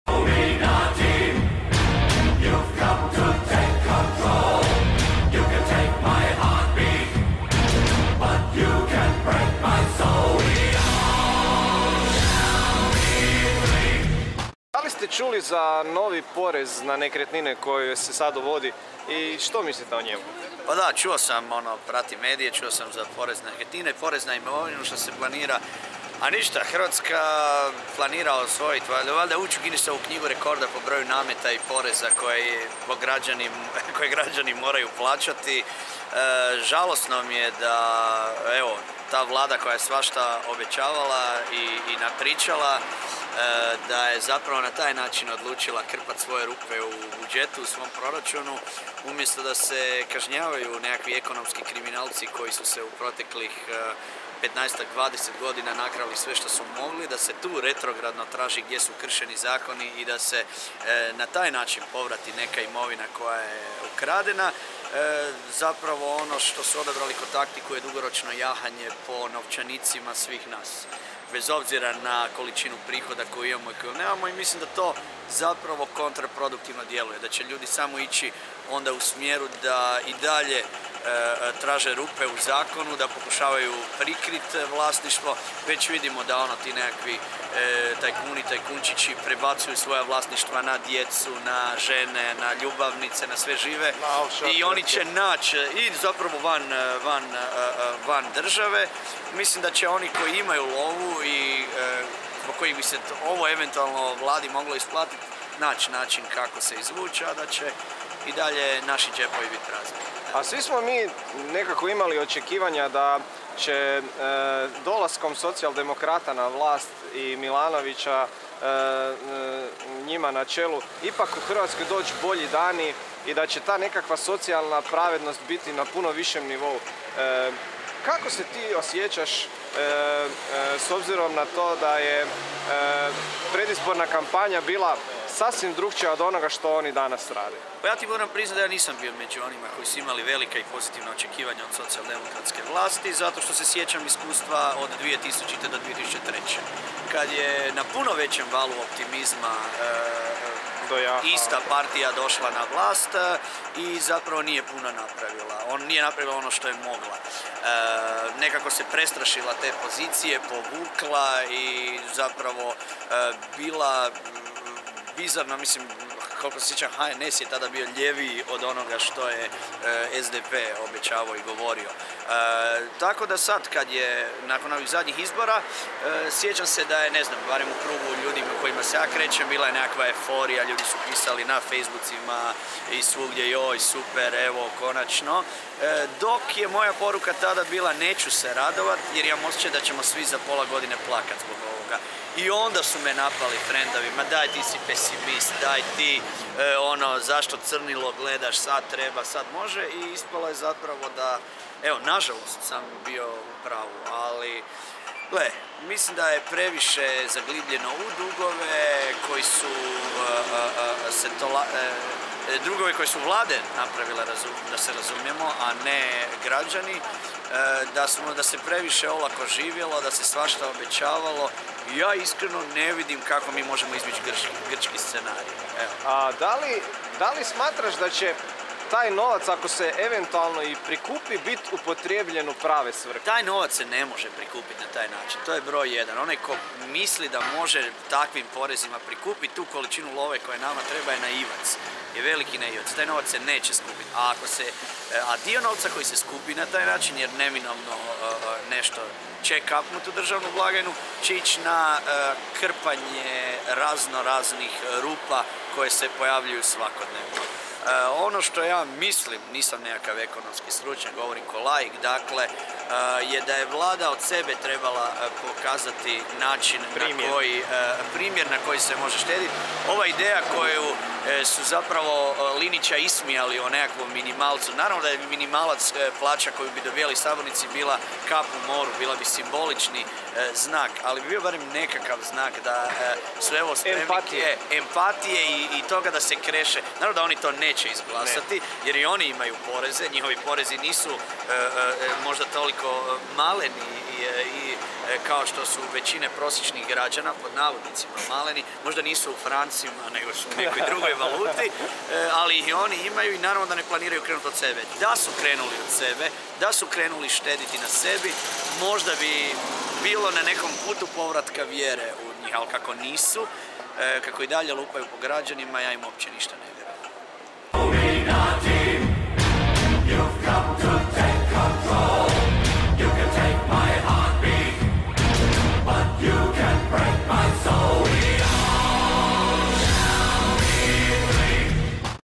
Mario Kovac
Sto poznati misle o Linicevom oporezivanju nekretnina i da li je SDP ispunio ocekivanja?

Govori Mario Kovač (Zagreb, 25. listopada 1975.), hrvatski kazališni i filmski redatelj. Kao režiser, glumac, kompozitor, scenograf i dramaturg sudjelovao je i u stvaranju čitavog niza amaterskih predstava i performansa. Djeluje i kao dramski pedagog te voditelj brojnih dramskih radionica.